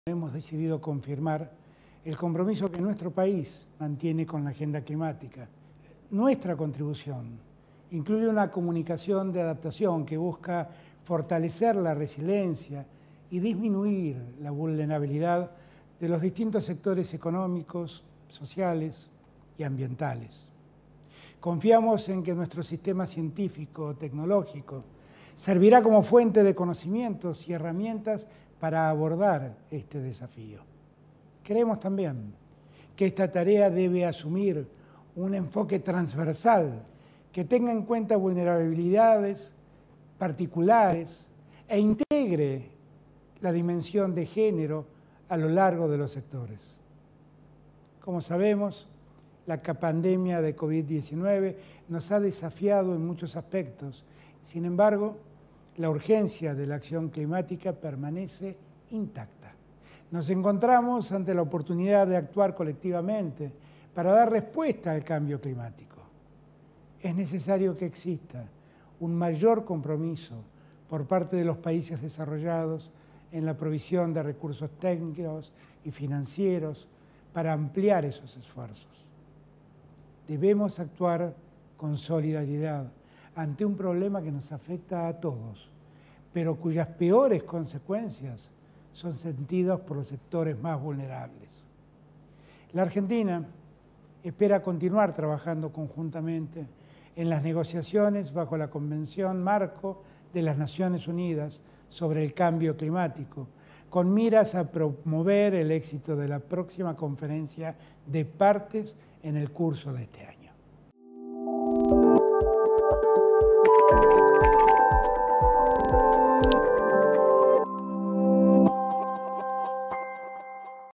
Alberto: "Decidimos confirmar el compromiso de nuestro país con la agenda climática"
El presidente Alberto Fernández sostuvo hoy que Argentina confirma su "compromiso" con la "agenda climática" con miras a "fortalecer la resiliencia y disminuir la vulnerabilidad de los distintos sectores económicos, sociales y ambientales".
Al participar de la Cumbre de Adaptación Climática 2021 que lleva a cabo la Organización de Naciones Unidas y que se realiza bajo la modalidad de teleconferencia a raíz de la pandemia de coronavirus, el primer mandatario dijo que el desafío necesita de un "enfoque transversal" que tenga en cuenta "vulnerabilidades particulares" y entre ellas mencionó especialmente "la dimensión de género".